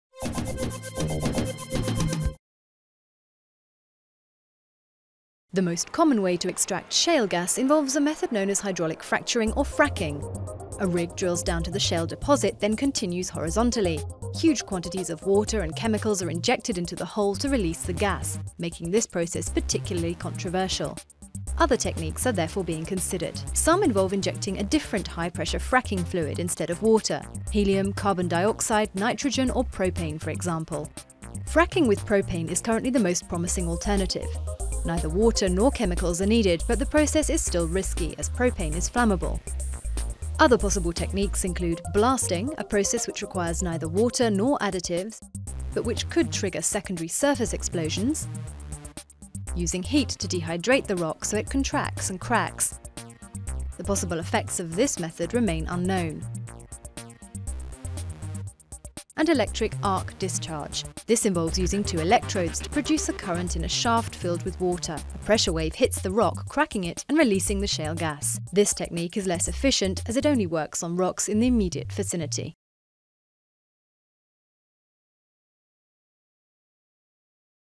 Shale gas extraction techniques
Germany has drawn up rules  on the hitherto unregulated technology of "fracking", narrowly restricting its use, but stopping short of an outright ban. Fracking or hydraulic fracturing is a process used to extract shale gas by blasting a high-pressure mixture of water, sand and chemicals deep underground to release hydrocarbons trapped between layers of rock.VIDEOGRAPHIC